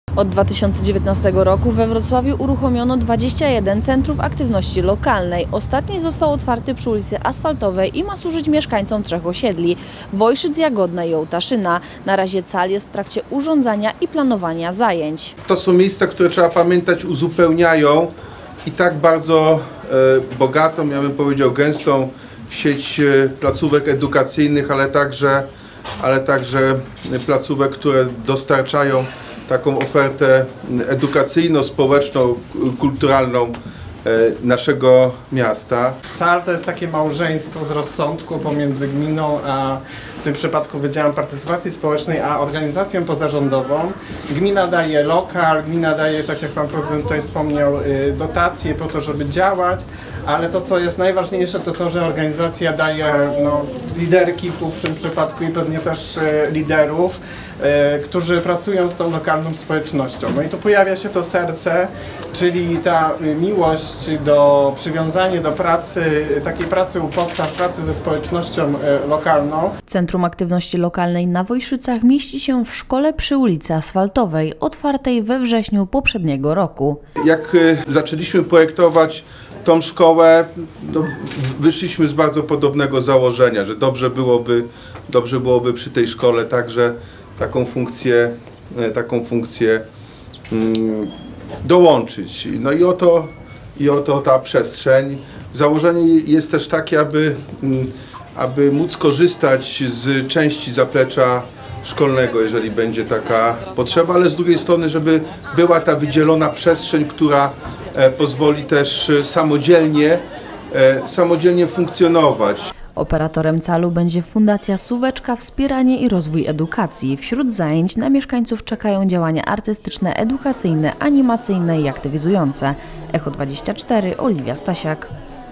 CAL na Wojszycach otwarty. Ma służyć trzem osiedlom
Od 2019 roku we Wrocławiu uruchomiono 21 Centrów Aktywności Lokalnej. Ostatni został otwarty przy ulicy Asfaltowej i ma służyć mieszkańcom trzech osiedli: Jagodna, Wojszyc i Ołtaszyna. Na razie CAL jest w trakcie urządzania i planowania zajęć.

Centrum Aktywności Lokalnej na Wojszycach mieści się w szkole przy ulicy Asfaltowej, otwartej we wrześniu poprzedniego roku.

Operatorem CAL-u będzie Fundacja Sóweczka Wspieranie i Rozwój Edukacji. Wśród zajęć na mieszkańców czekają działania artystyczne, edukacyjne, animacyjne i aktywizujące.